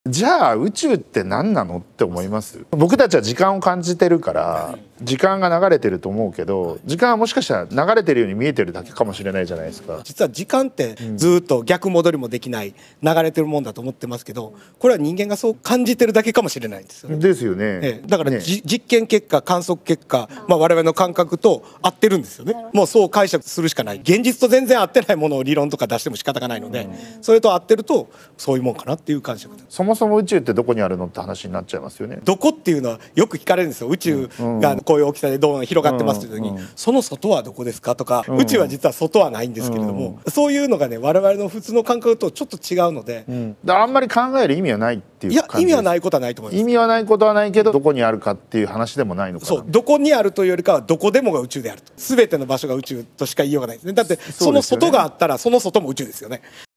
【堀江貴文】宇宙の謎に"物理学の異端児"が答える
HORIE ONE：宇宙の謎を解け「ニュートリノ研究」最前線【ゲスト：多田将】